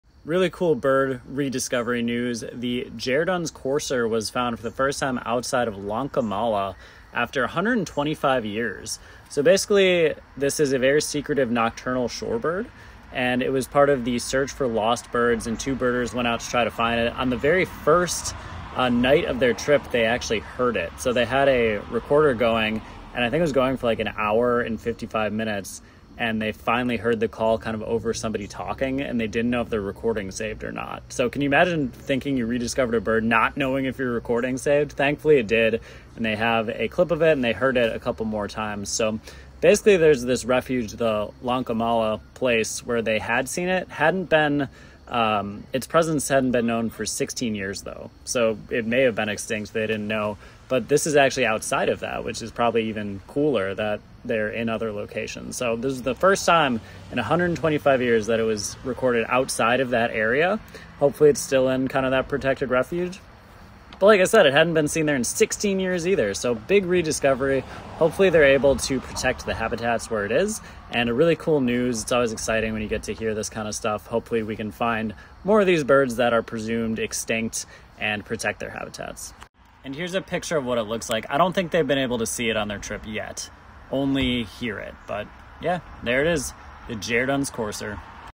Breaking Bird Re-Discovery News! 🚨
Two birders, Harish Thangaraj and Adesh Shivkar, detected Jerdon’s Courser outside Lankamalla for the first time in 125 years—and on the very first night of their 2025 trip. This is also the first confirmed record in 16 years, proving the species survives beyond its tiny known range and offering new hope for conservation.